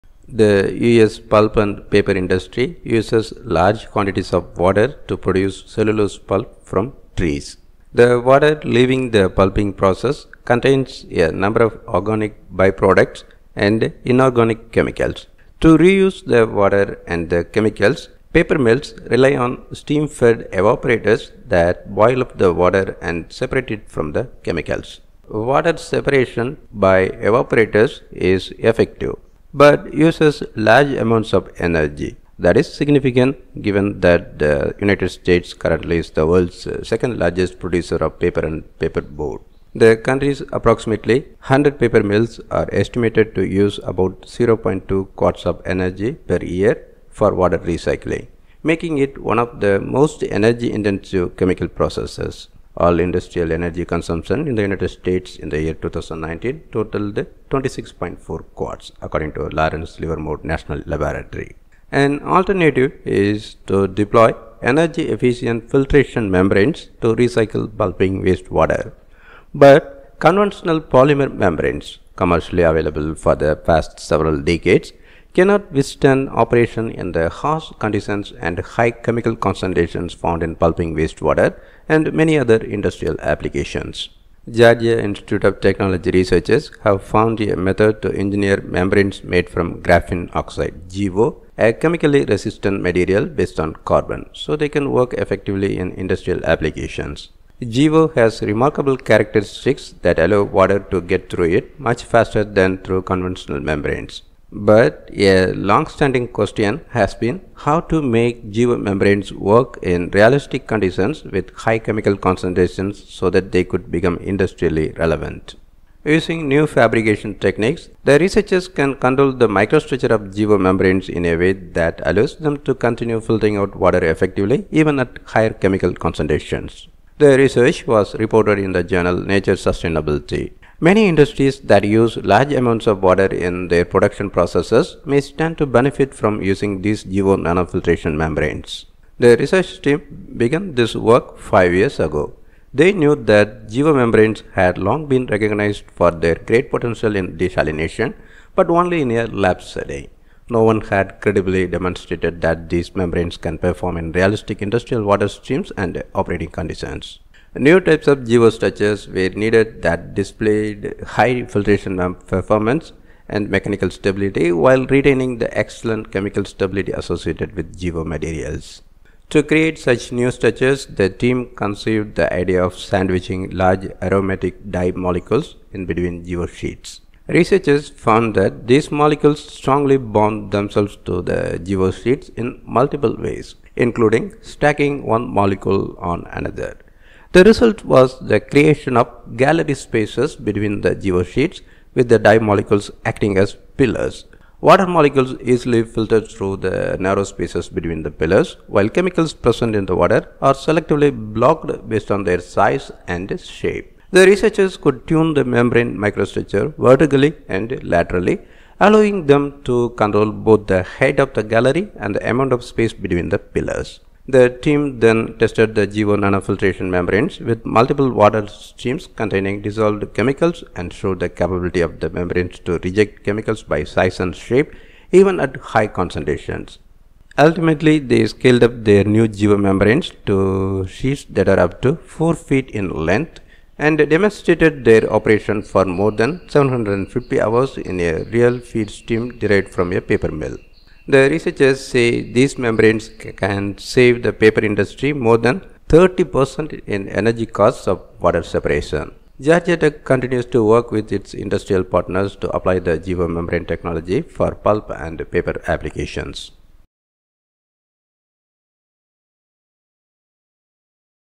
Graphene Oxide membranes could reduce paper industry energy costs
Georgia Institute of Technology researchers have found a method to engineer membranes made from graphene oxide (GO), a chemically resistant material based on carbon, so they can work effectively in industrial applications.

Credit: NEFLO Photo/Shutterstock.com
Credit: Georgia Tech

Research paper: Graphene oxide nanofiltration membranes for desalination under realistic conditions